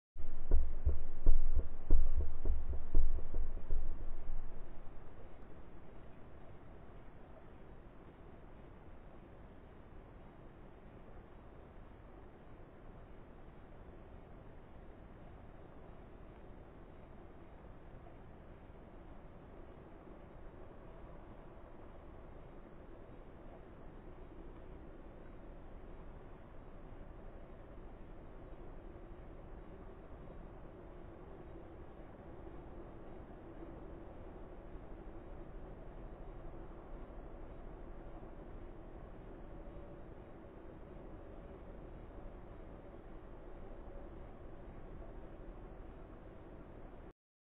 Gopro hero3+ in slow motion test
Este video es una prueba en slow motion y fue grabado en 720p 100fps